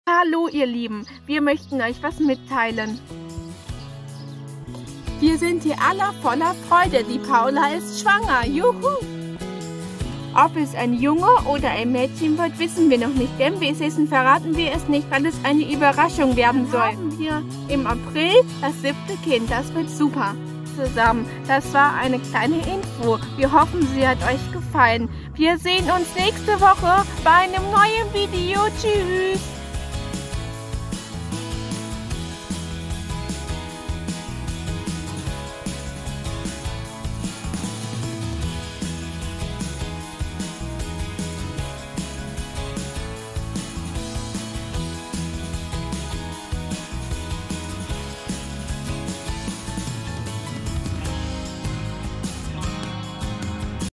Familie Schmuckkörbchen eine kleine Info
Hallo zusammen
Die Schmuckkörbchen ist Voller Freude Paula ist Schwanger.
Im April wird Baby 7 dabei sein.

Liebe Grüße
Eure Familie Schmuckkörbchen